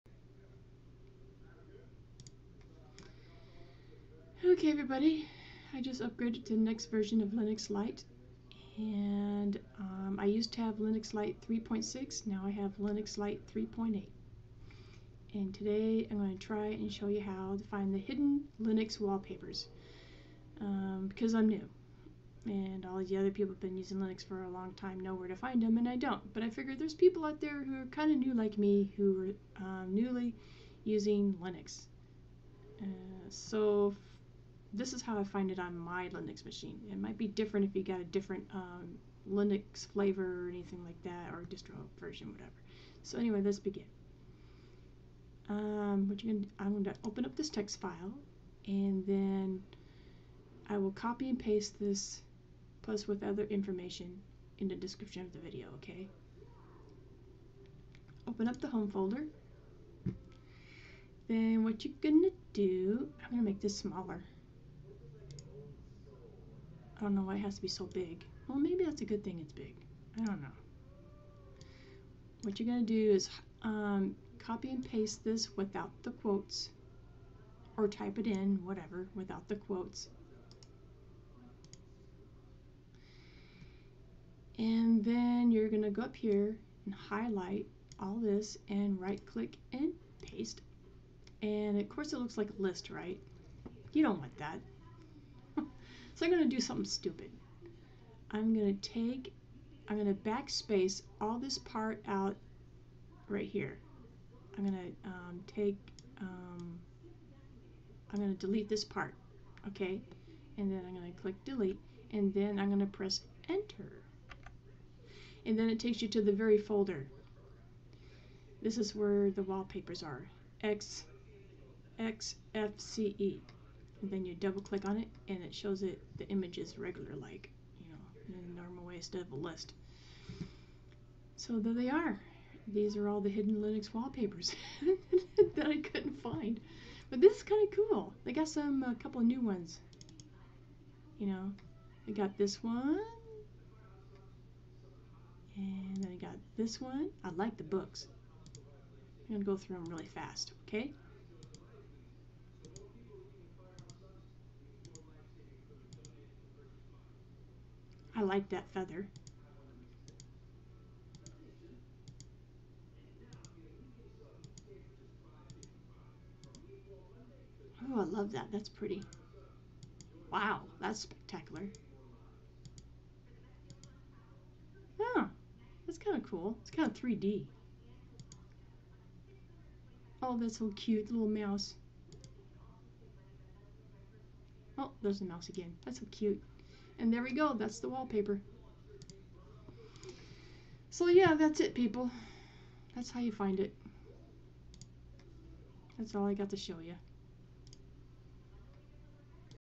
Linux lite 3.8 : How to find the hidden linux wallpaper folder
you know how you start using your own images as wallpaper, then for some reason you can no longer find the Linux wallpaper folder , for their cool wallpaper that came with the Linux version you installed on your old PC ?, well this video i show you how to find it .

here is what you copy and paste with out the quotes, " /usr/share/backgrounds/xfce/ "